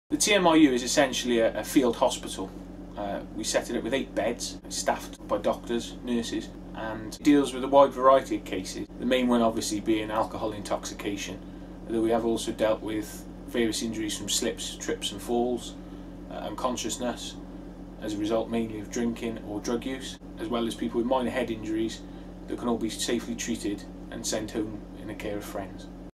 WMAS Temporary Minor Injuries Unit
The 18th, 19th and 31st December are expected to be the busiest festive party nights for West Midlands Ambulance Service. Once more, it's Temporary Minor Injuries Unit (TMIU) will be operating near Broad Street in Birmingham and in this video Paramedic Mike Duggan explains what it's for.